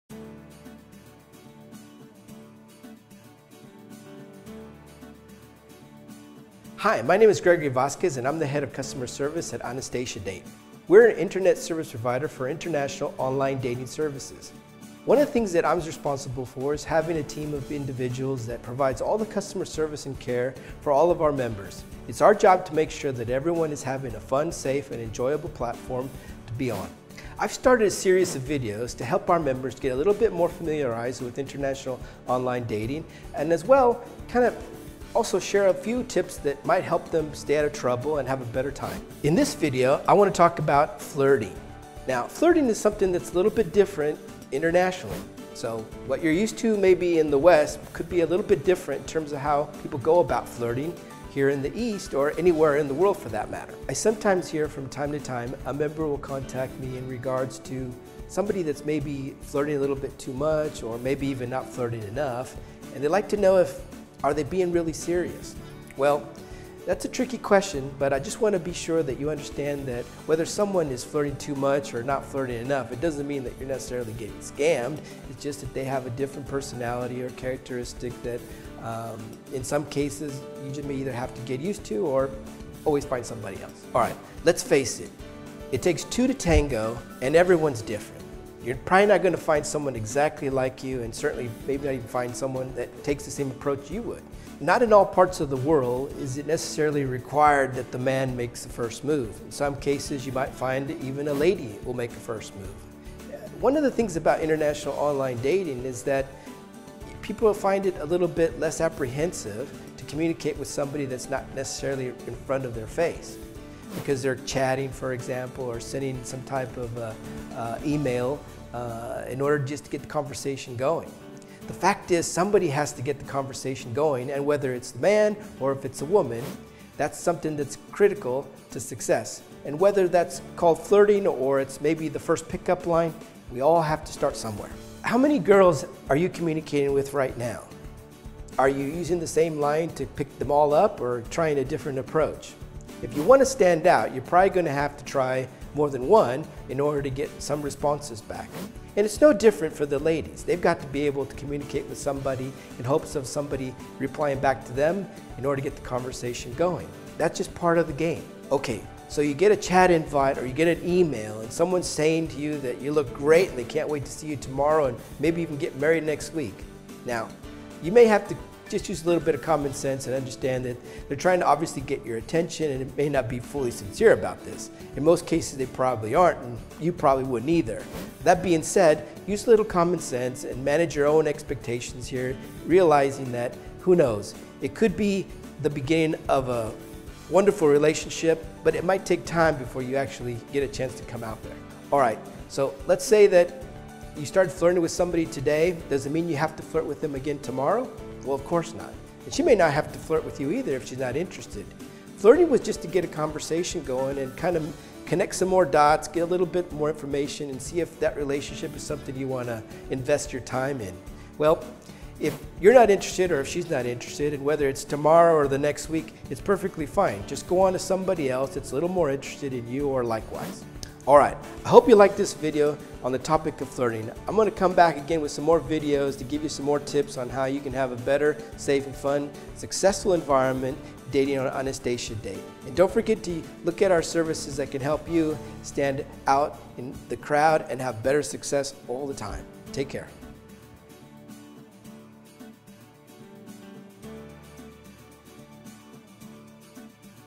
AnastasiaDate: How to flirt online?
In this video I will be covering the topic of flirting online and how it may take different forms internationally.
I sometimes hear from our site’s members that some people flirt more or less and if there is a way to know if someone is just flirting or really serious.  The reality is that this is a tricky question to answer.

Flirting is one way to make an initial move.  And just like it is real life…how and what that looks like is up to anyone to decide.  For some members it might be by simply saying “hello”.

For others it might be something more interesting like a compliment “you have such beautiful eyes”.  And yet for others it might even be more direct and somewhat “adult in nature” if you know what I mean.

Flirting by paying someone a compliment is often a safe way to get a conversation started.  However please note that the aim of starting a conversation was the real goal here.  The compliment itself was just a vehicle to get attention and see what kind or response you get.